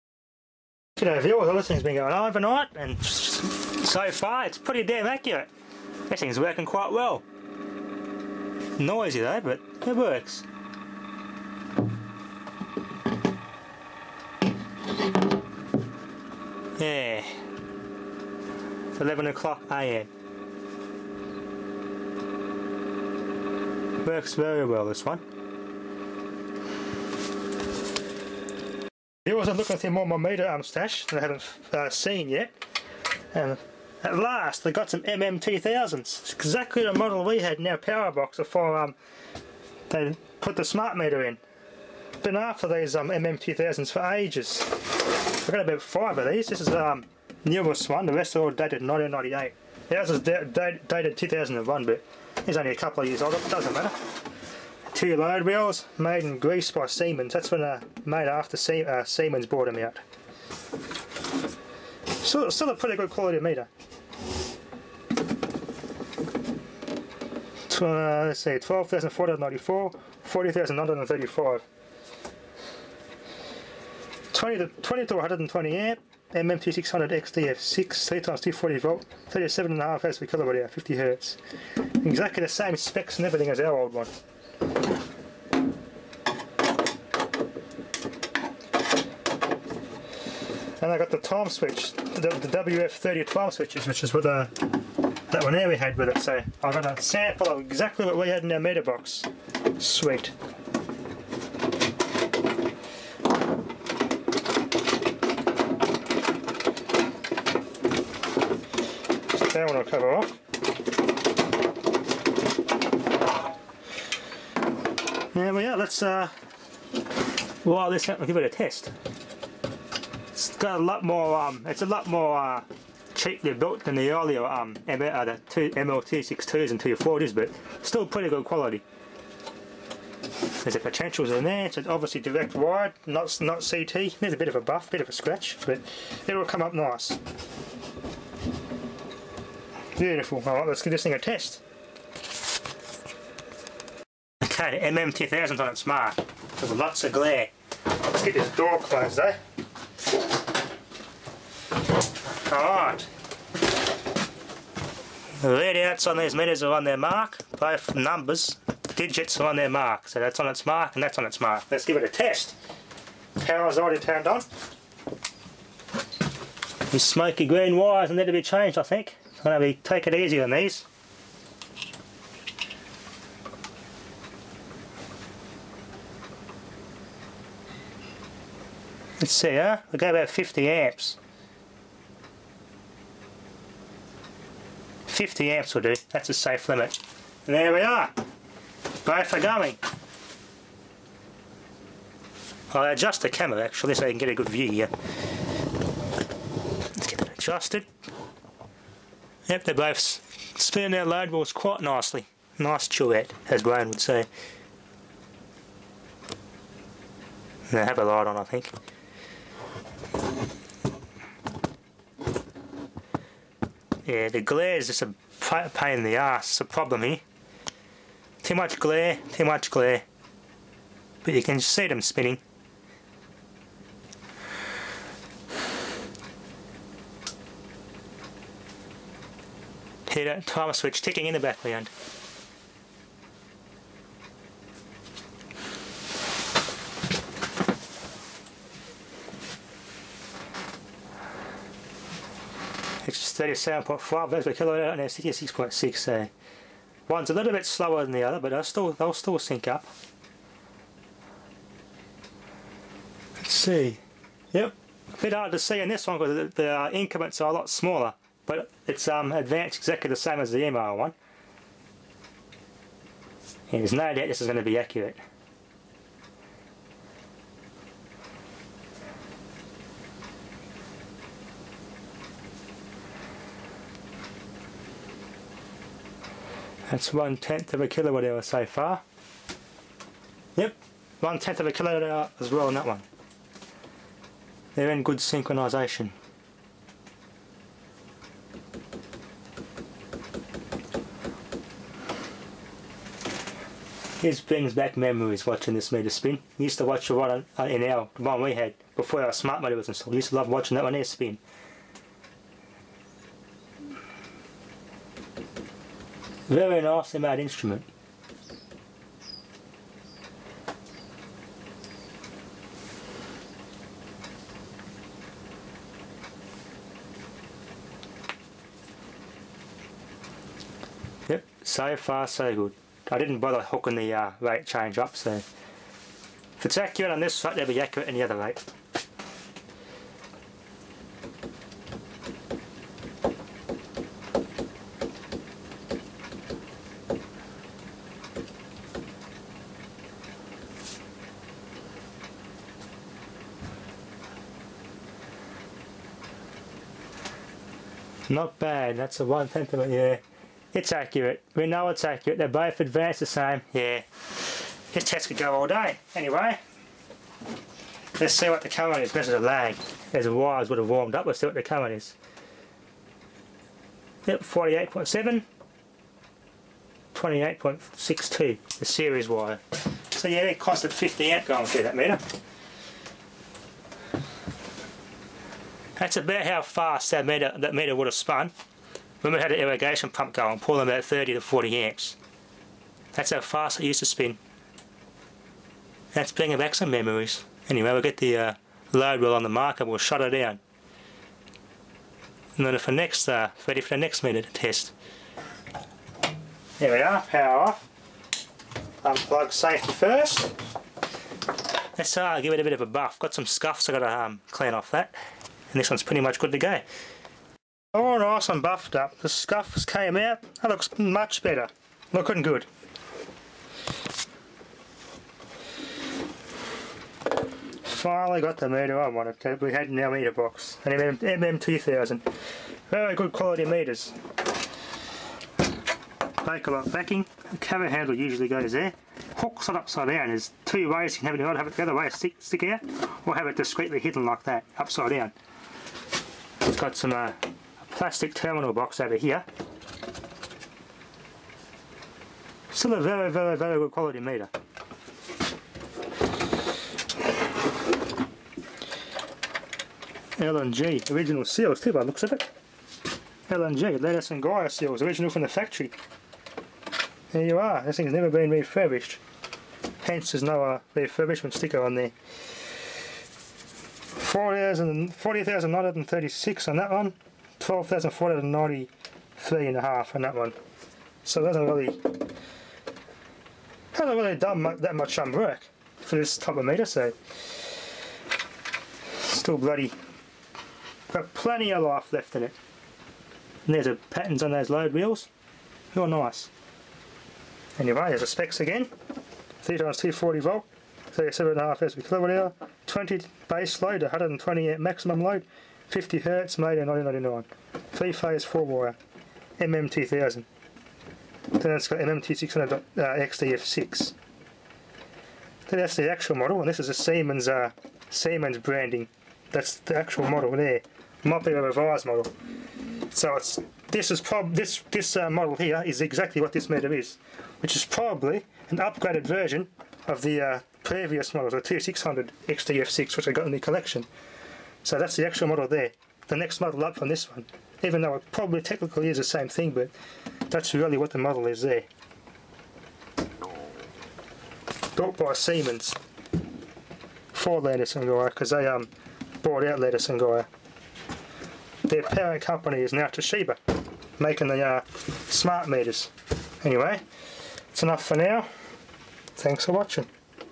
Siemens Landis & Gyr MM2000 Watthour Meter Test
Finally Got some of these!! Cleaned up well, Characteristics are: 3 Phase 3x 240V, 20 (120)Amp, 37.5Rev/Kwh, 50Hz